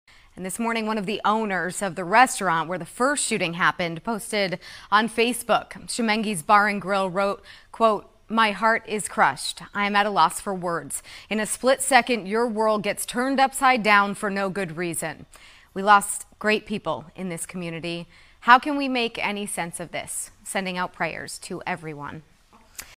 Maine restaurant hit by mass shooting shares post on social media
Leaders from a restaurant at the center of a mass shooting in Lewiston, Maine, shared a message overnight on social media.